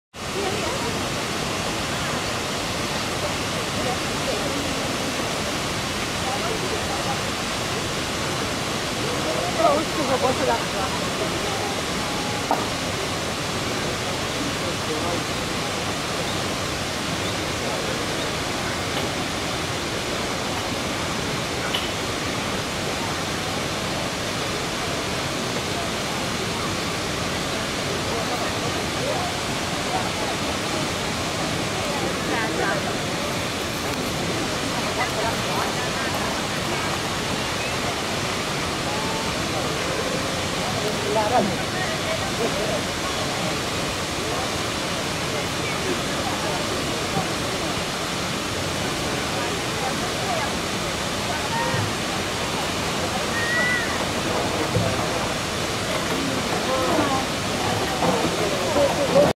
Cheonjiyeon Waterfall, Jeju Island
Cheonjiyeon Waterfall is a waterfall on Jeju Island, South Korea. Its name means sky connected with land and this is one of the main tourist attractions on Jeju-do,